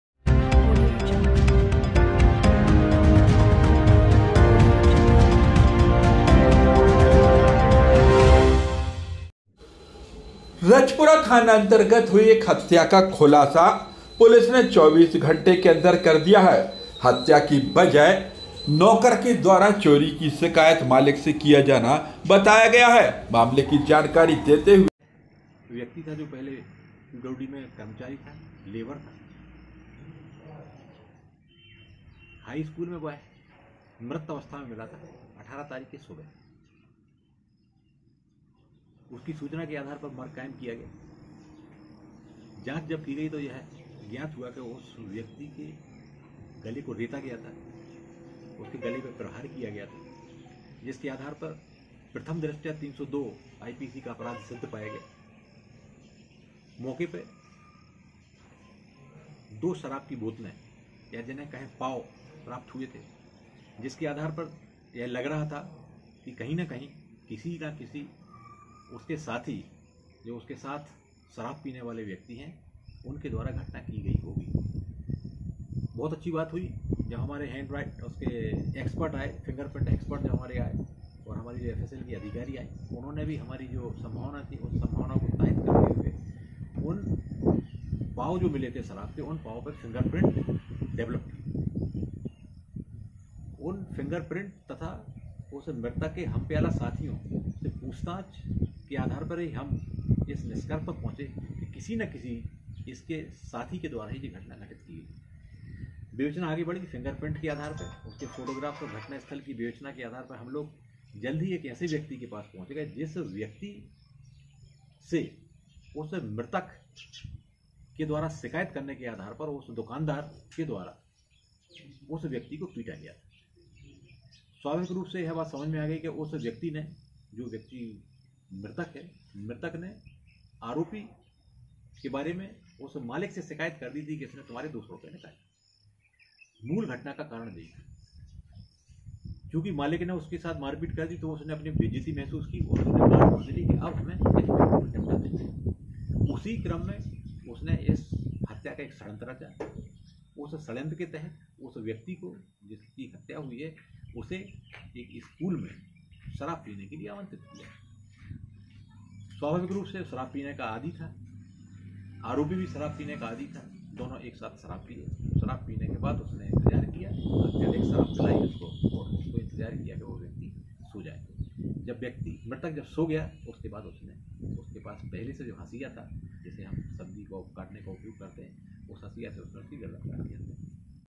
मालिक से नौकर की चोरी की चुगली महंगी पड़ी.. रजपुरा क्षेत्र मे अंधी हत्या का एसडीओपी ने किया खुलासा..
रजपुरा थाना अंतर्गत पिछले दिनों स्कूल के पास कोई हत्या की गुत्थी को पुलिस ने 24 घंटे में सुलझा लिया है मामले में आरोपी और कोई नहीं बल्कि मृतक का दोस्त निकला है जिसके द्वारा उसके मालिक से ₹200 चोरी की शिकायत की गई थी इसी पर से उसने उसे मौत के घाट उतार दिया था